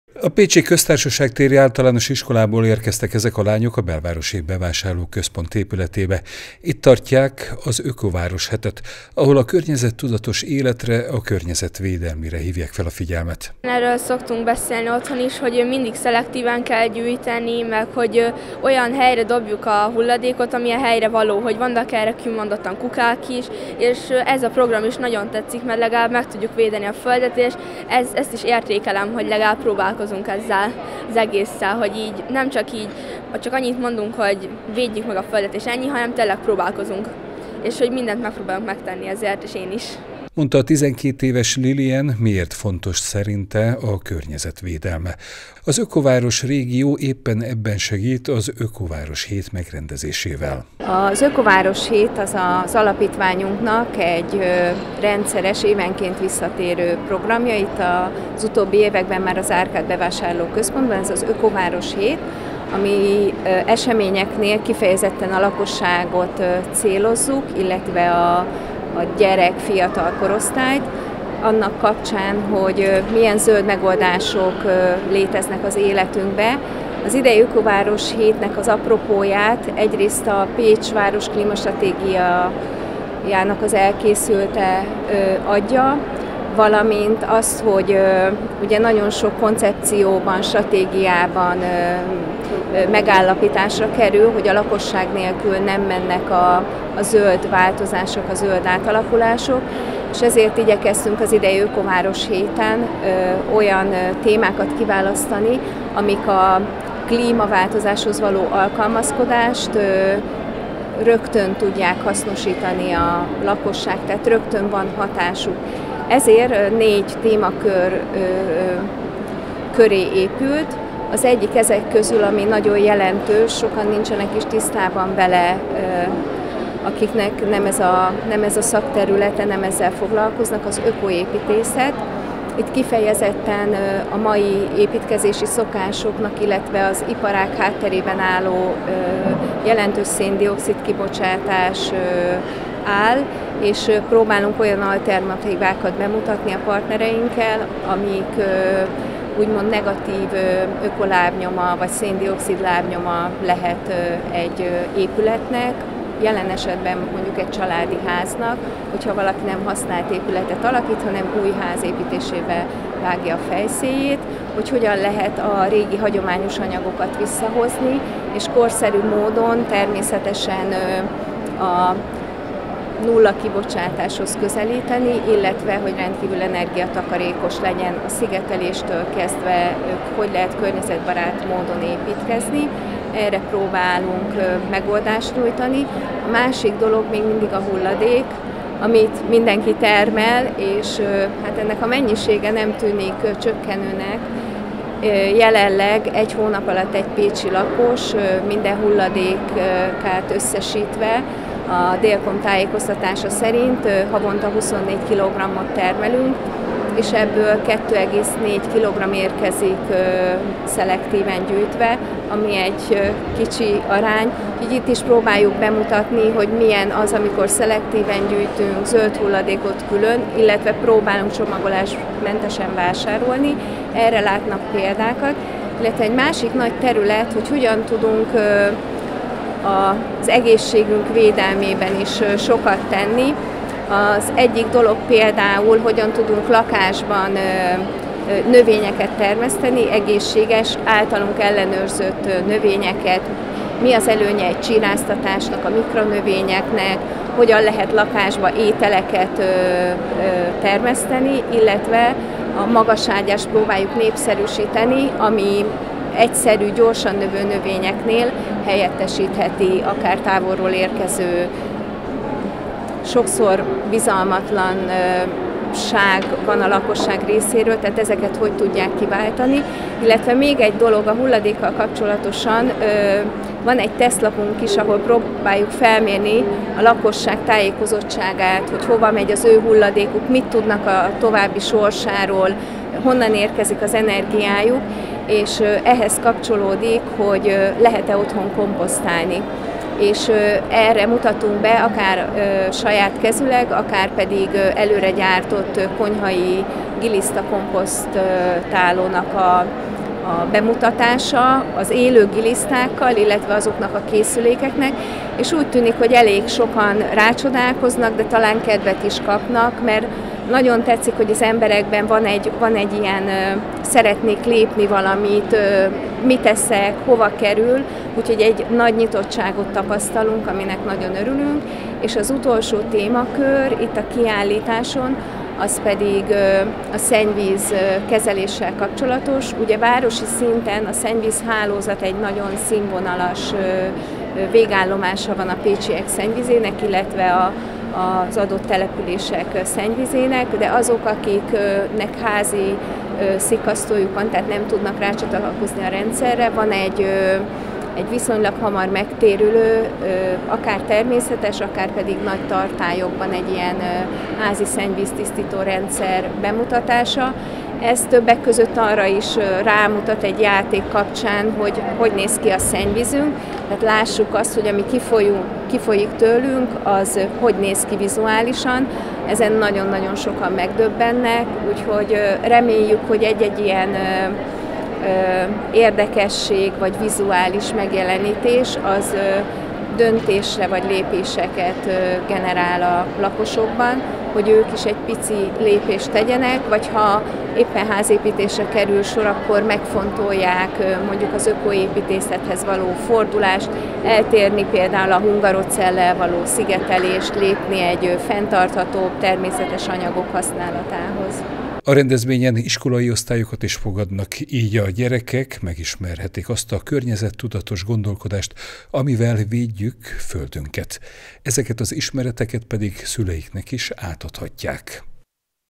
Otthonunkban is tehetünk a Földért - Ökováros Hetet tartanak Pécsen
Milyen zöld megoldásokat alkalmazhatunk otthon, amelyekkel védjük bolygónkat? Ezekről is hallhatnak azok az érdeklődők, akik ellátogatnak a pécsi belvárosi üzletközpontba. Itt tartja az Ökováros Hét rendezvényét az Ökováros-Ökorégió Alapítvány. Idén azt szeretnék bemutatni, hogy apróbb változtatásokkal is környezettudatosabb életet élhetnek az emberek.